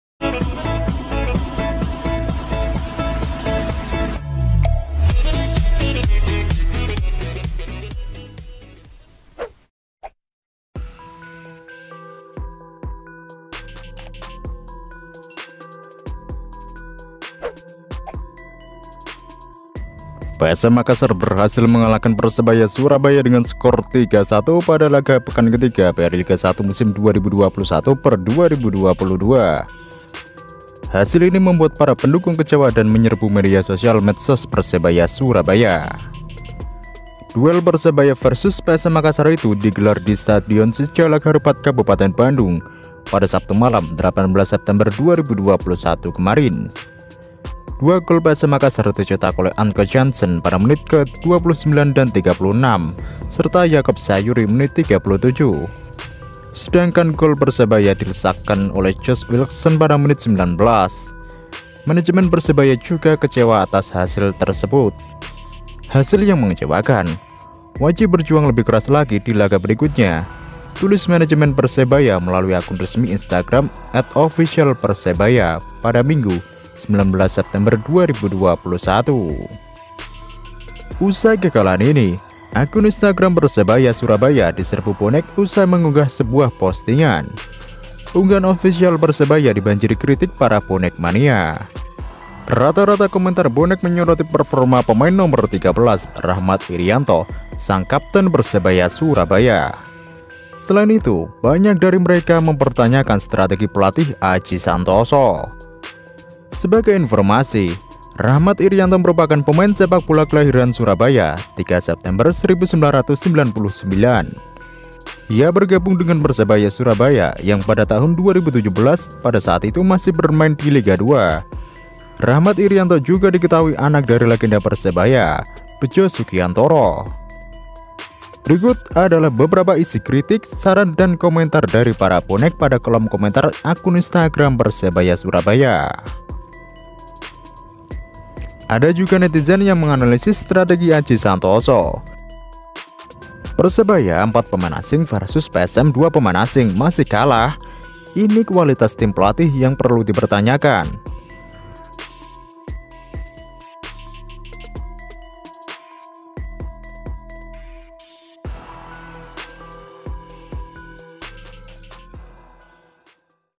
Usai Dikalahkan PSM Makassar, Akun IG Persebaya Diserbu Bonek, Soroti Pemain No 13 dan Aji Santoso
Untuk kalian yang suka video Ini, bisa like dan share video ini.

By: Suara Bola